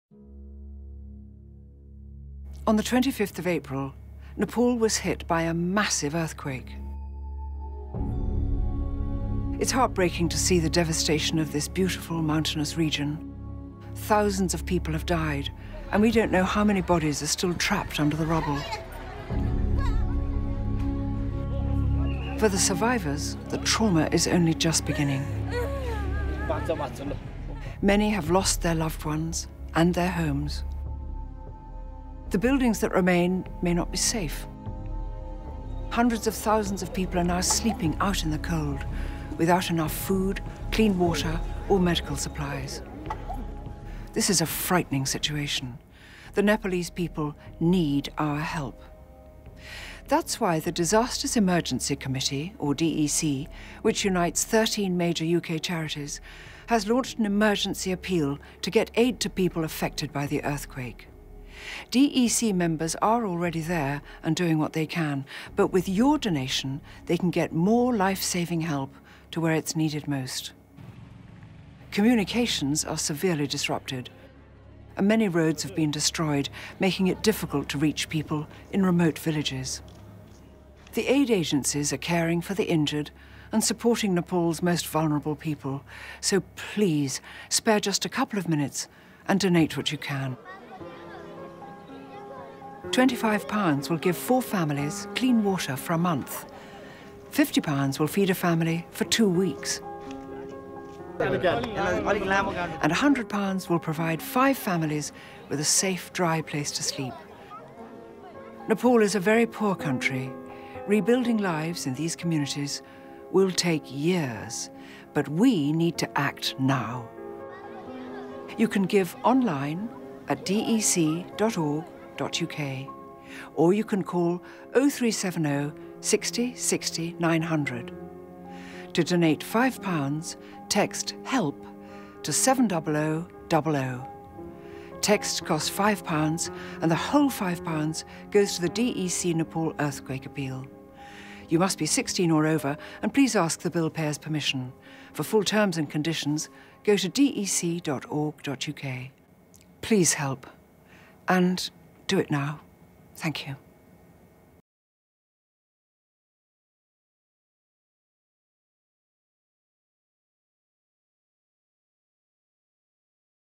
BBC Nepal Earthquake Appeal
Over 5000 people have died and 8 million more need urgent help due to the devastation caused by the Nepal earthquake. Please give all you can and we'll ensure our member agencies get the help to those in urgent need.